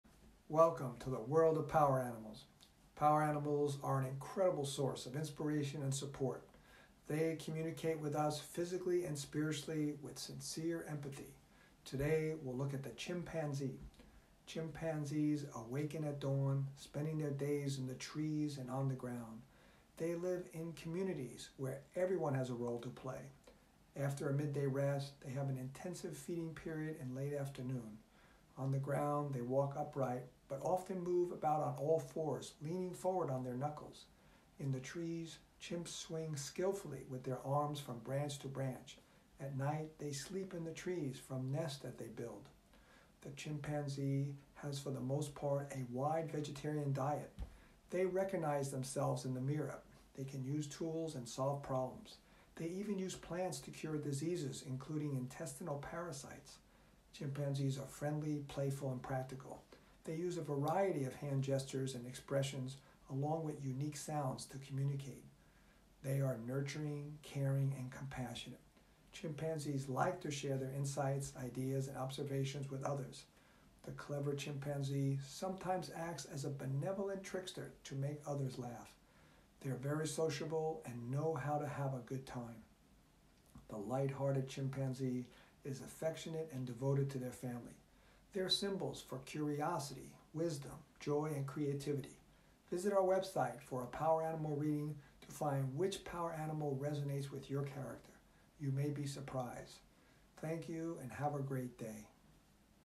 The Chimpanzee Power Animal
Welcome to the world of power animals. Power animals are an incredible source of inspiration and support. They communicate with us physically and spiritually with sincere empathy. Today we’ll look at the chimpanzee.

Chimpanzees awaken at dawn, spending their days in the trees and on the ground. They live in communities where everyone has a role to play. After a midday rest, they have an intensive feeding period in the late afternoon. On the ground, they walk upright, but often move about on all fours, leaning forward on their knuckles. In the trees, chimps swing skillfully with their arms from branch to branch. At night they sleep in the tree from nests that they build. The chimpanzee has for the most part a wide vegetarian diet. They recognize themselves in the mirror, can use tools, and solve problems. They even use plants to cure diseases including intestinal parasites.

Chimpanzees are friendly, playful, and practical. They use a variety of hand gestures and facial  expressions along with unique sounds to communicate. Chimpanzees are nurturing, caring, and compassionate.  They like to share their insights, ideas, and observations with others. The clever chimpanzee sometimes acts as a benevolent trickster to make others laugh. They’re very sociable and know how to have a good time.

The light-hearted chimpanzee is affectionate and devoted to their family. They’re symbols for curiosity, wisdom, joy, and creativity. You may be surprised.